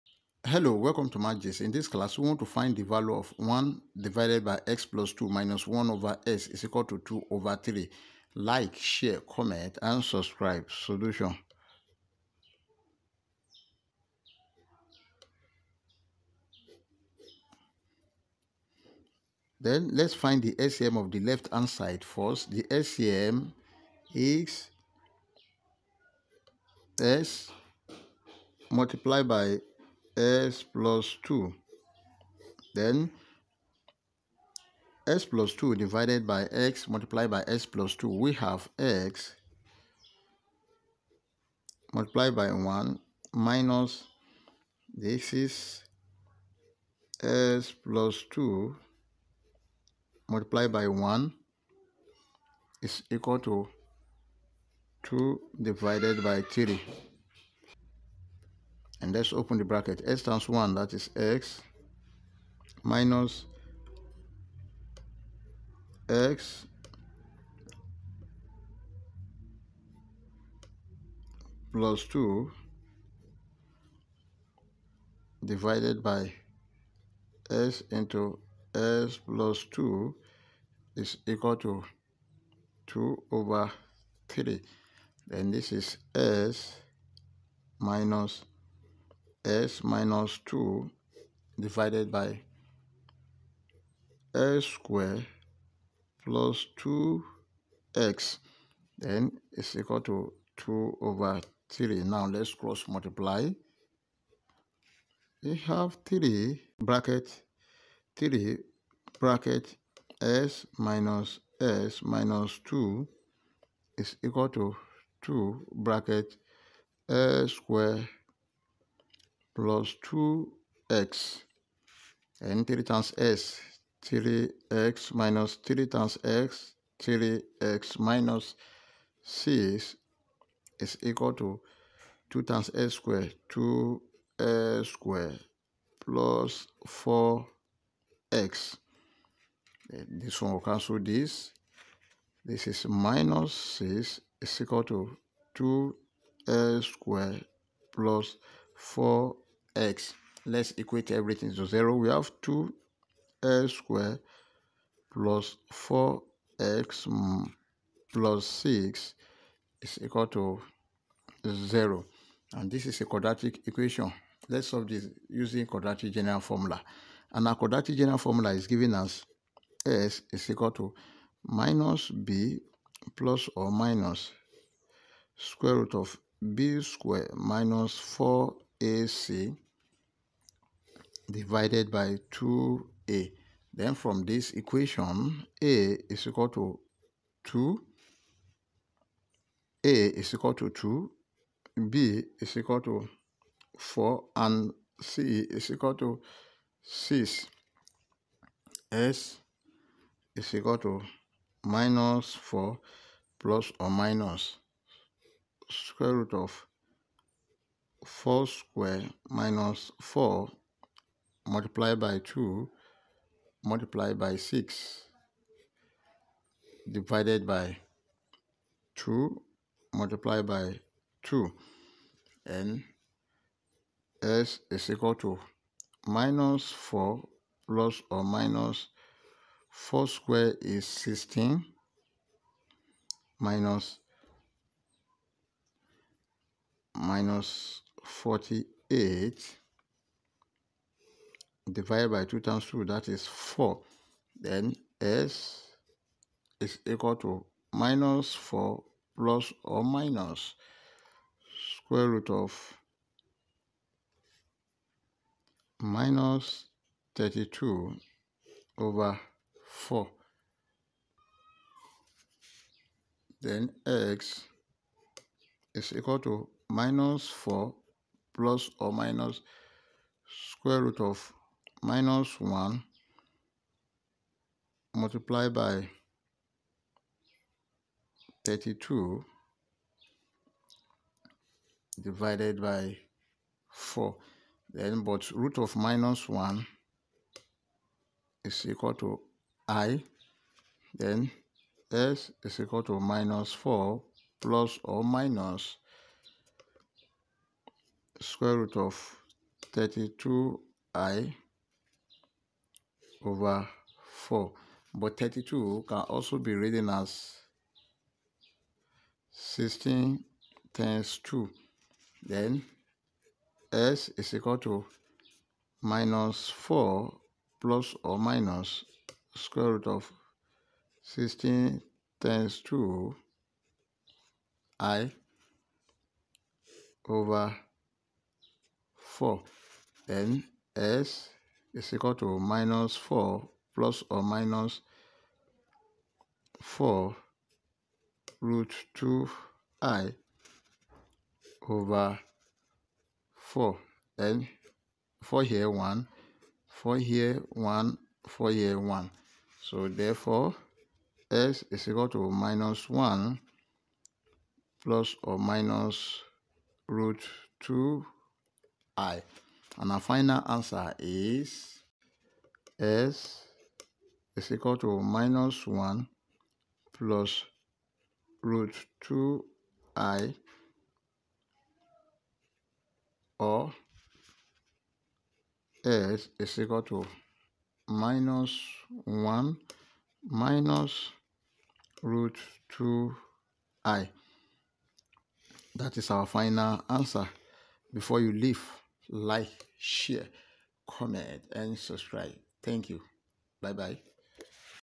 What Is The Value Of X In This Nice Algebra Equation?
This video will teach you how to solve the given nice algebra equation very fast with this truck.